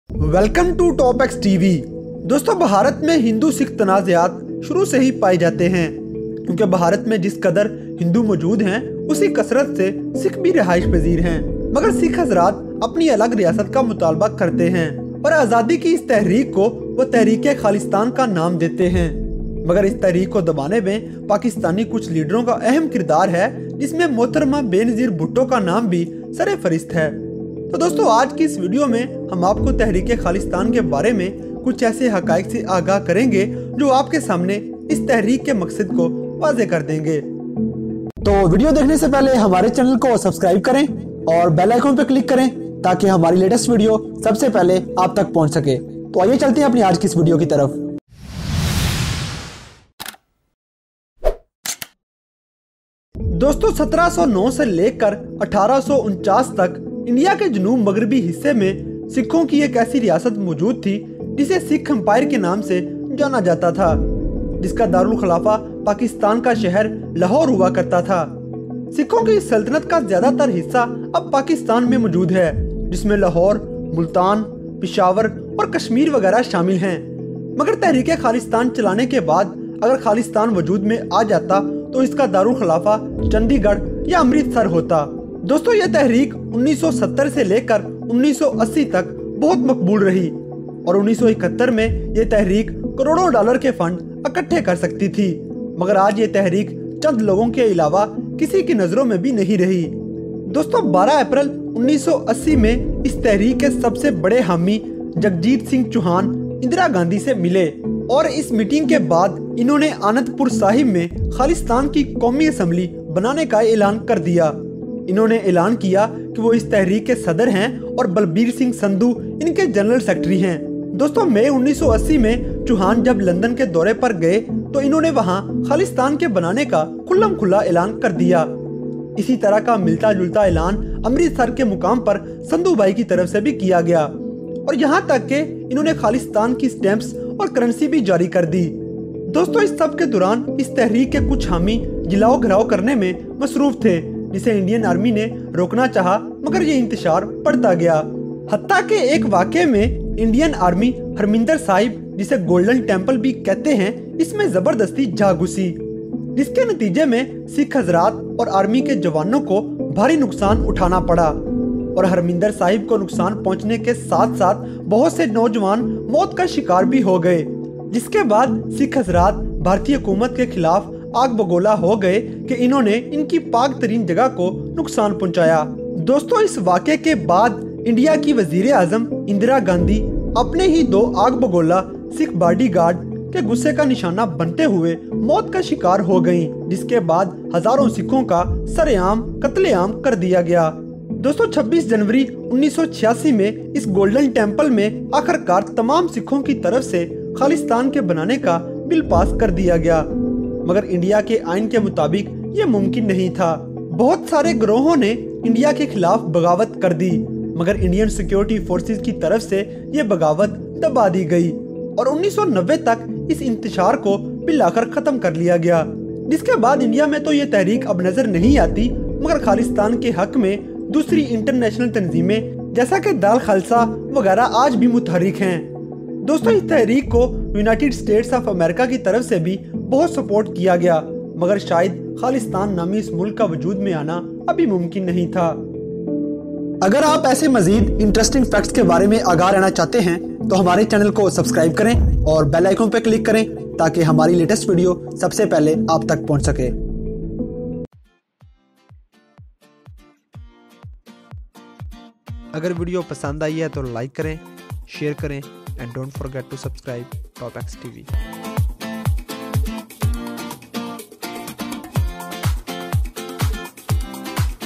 Amazing And Shocking Facts About Khalistan Movement | TOP X TV
Amazing And Shocking Facts About Khalistan Movement | Urdu / Hindi |

Hello friends, in this video we will be talking about Surprising Facts About Khalistan Movement. I hope you will like this video.